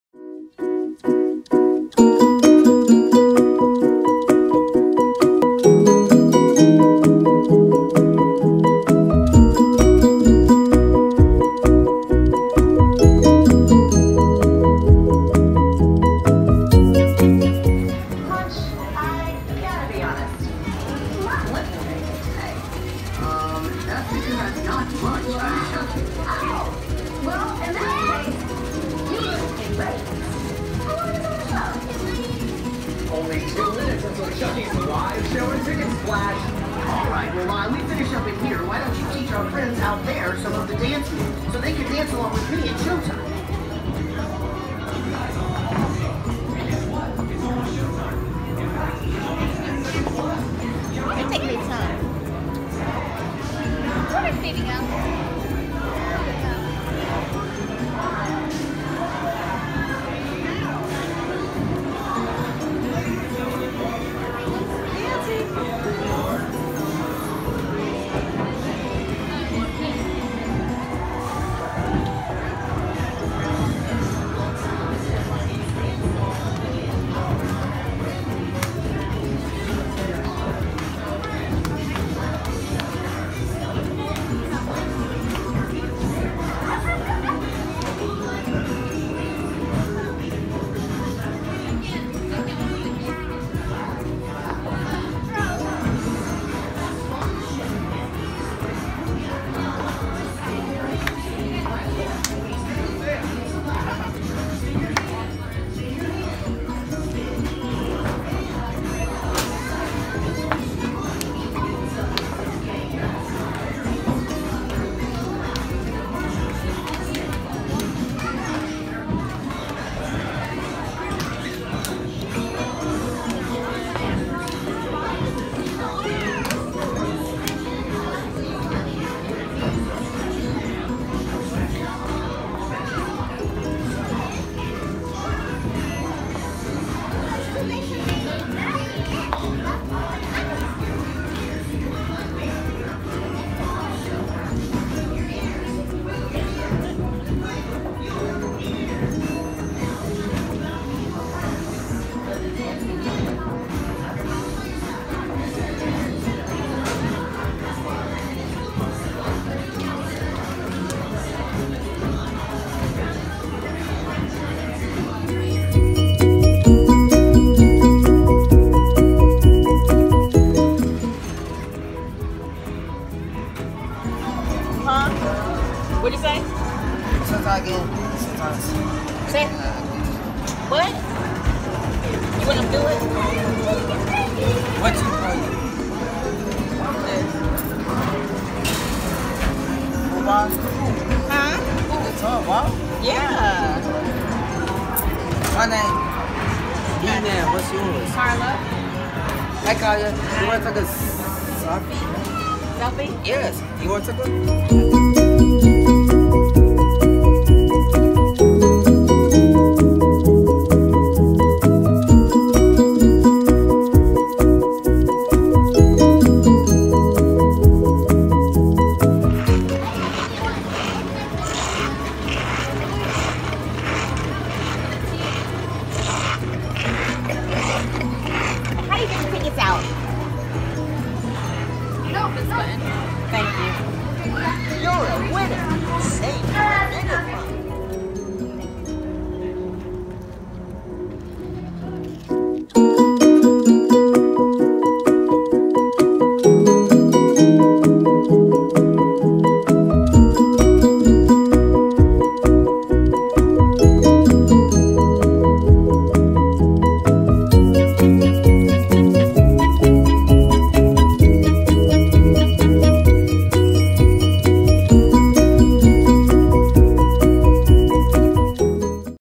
Chucky Cheese Clarksville Tennessee
The little boy thought I was slow because I was having fun